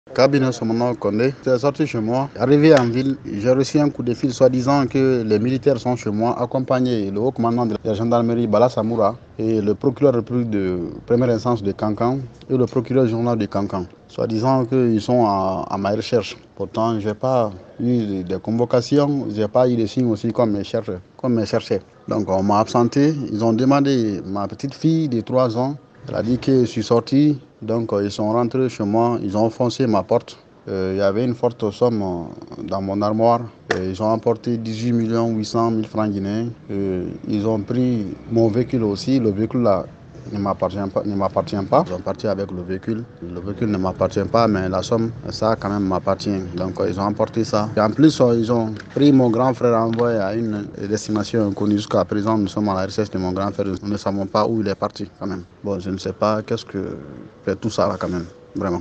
GUINÉE:KANKAN: UN CITOYEN DÉNONCE UNE VIOLATION DE DOMICILE, VOL ET ENLEVEMENT
Leguide.info : depuis le début des manifestations contre le délestage électrique à Kankan, les principaux meneurs sont inquiétés. Le dernier en date est Kabinet Somano Condé, qui nous informe que son domicile a été investi par des hommes en uniforme, à leur tête un haut gradé de la gendarmerie nationale. Suivons son récit.